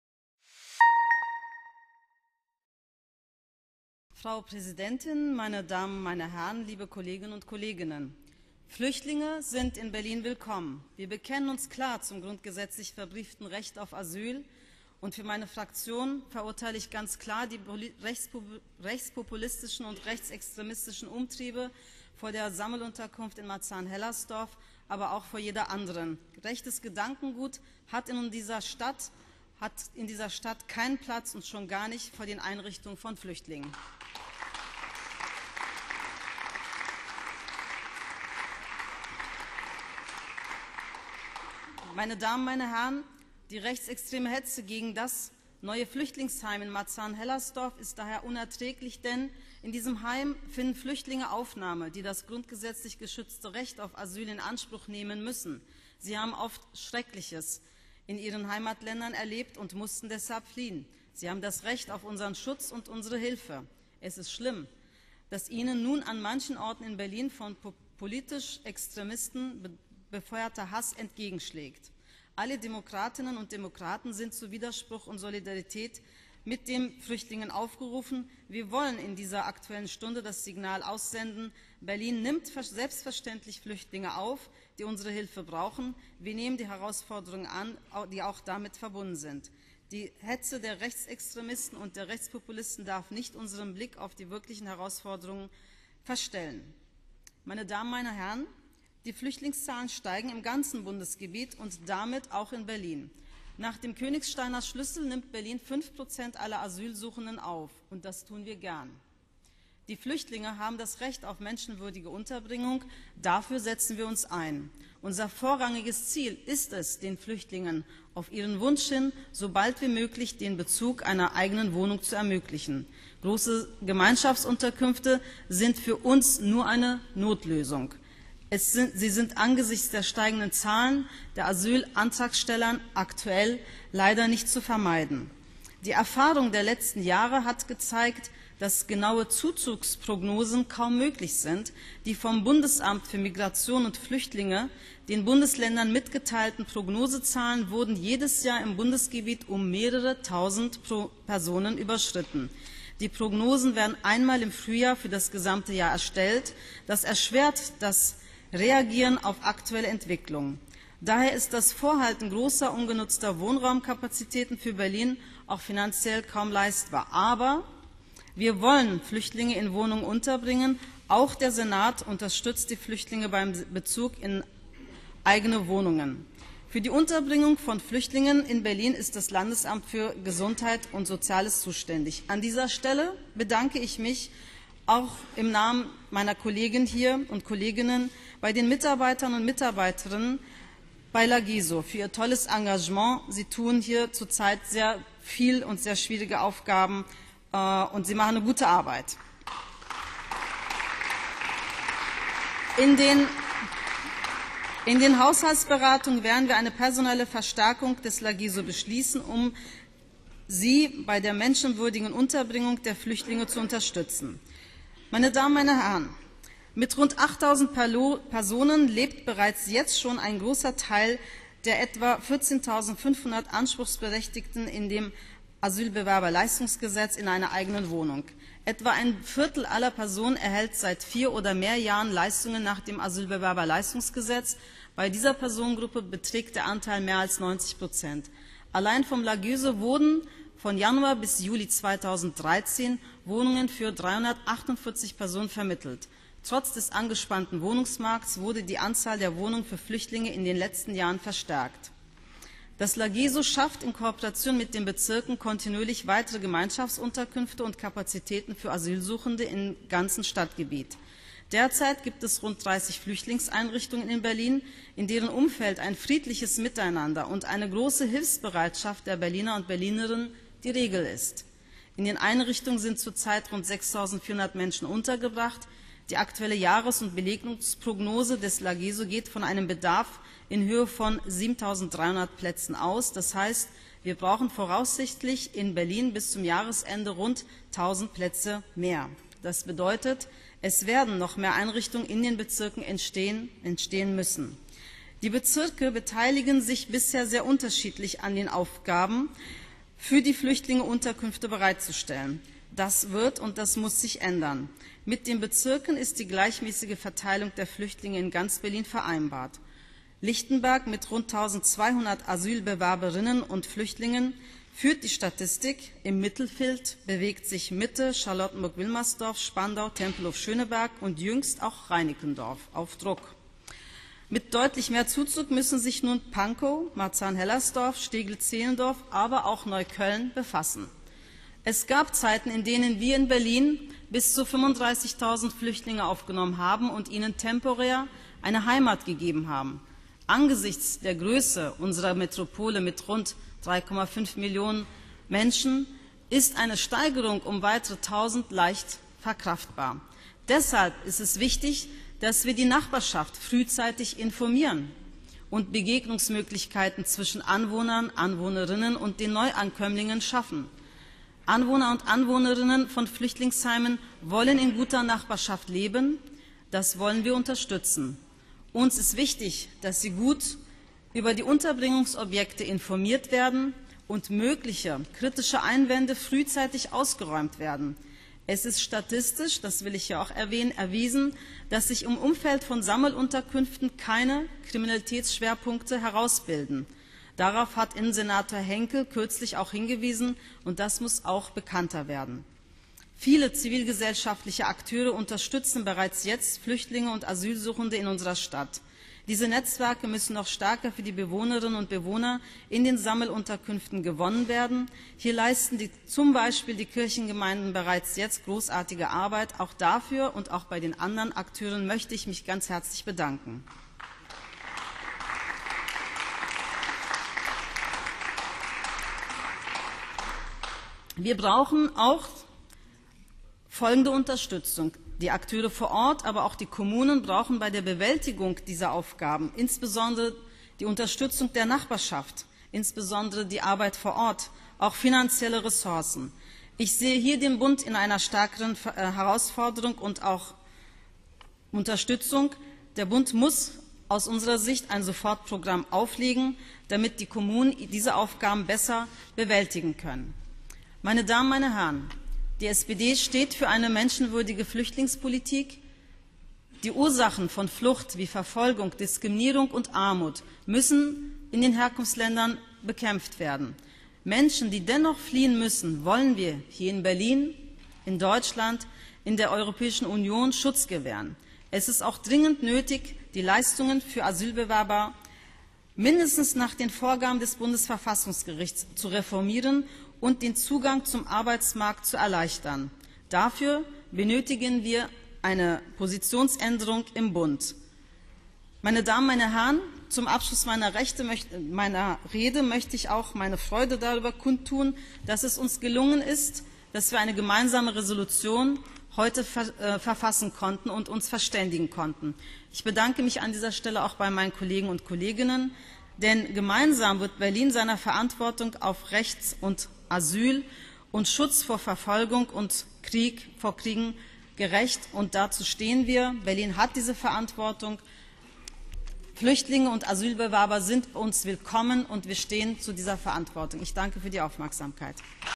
34. Sitzung des Berliner Abgeordnetenhauses - Aktuelle Stunde - Ülker Radziwill (SPD)
34. Sitzung des Berliner Abgeordnetenhauses - 29. August 2013
Aktuelle Stunde - Gemeinsam wird Berlin seiner Verantwortung gerecht: Recht auf Asyl und Schutz vor Verfolgung und Krieg
Ülker Radziwill (SPD)